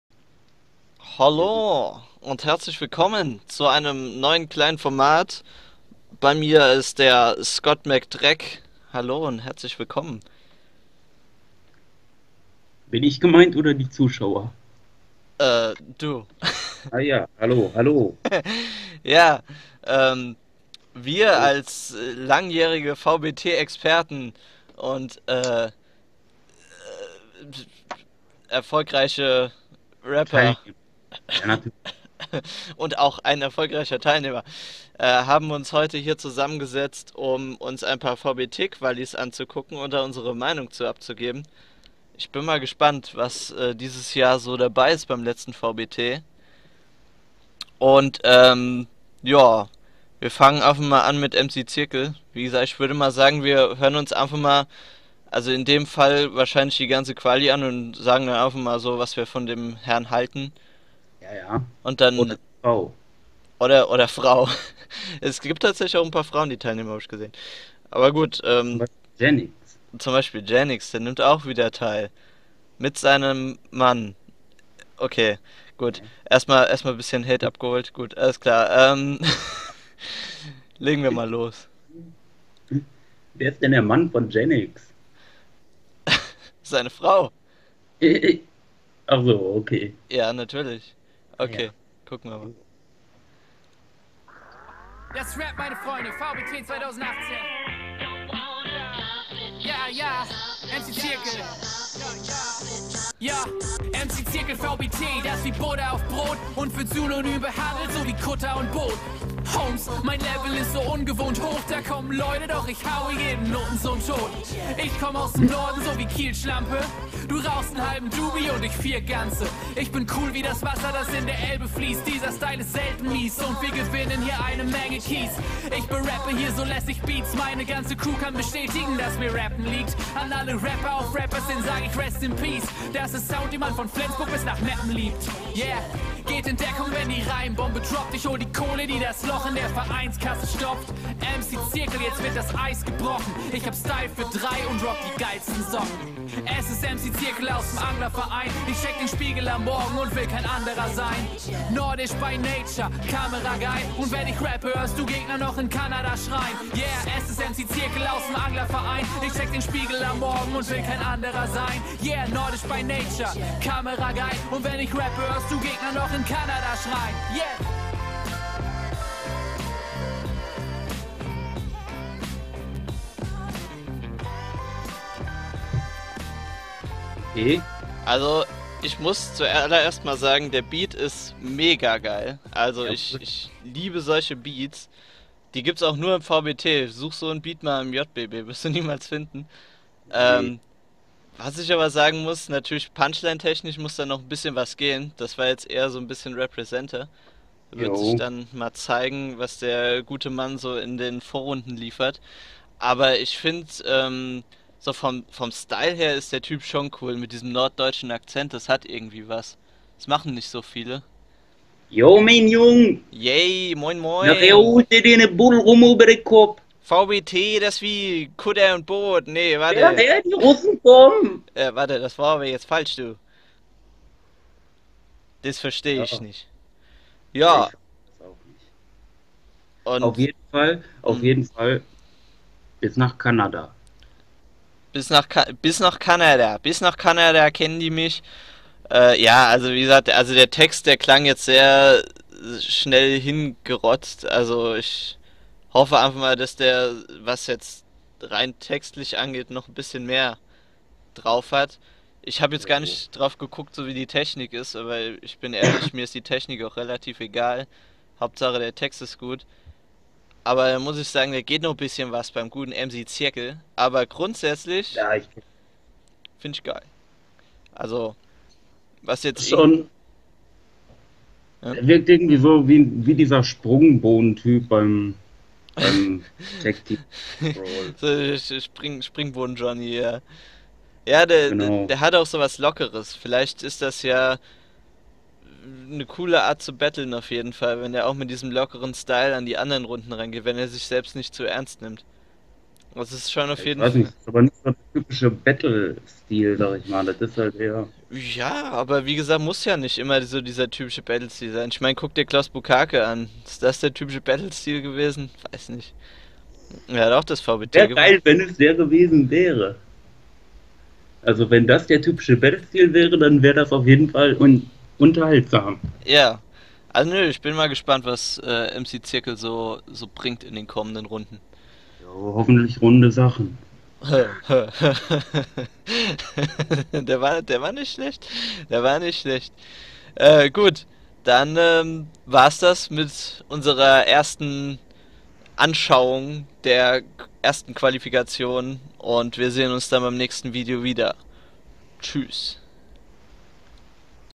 Lets Talk About VBT 2018 Qualifikation: MC ZIRKEL
Zu allererst sollte man sagen das wir oftmals nicht ALLES ernst meinen und besonders Skot den ein oder anderen scherz bringt. desweiteren möchte ich mich für die schlechte Bildquali und den abgeschnittenen Bildschirm während der Totale entschuldigen.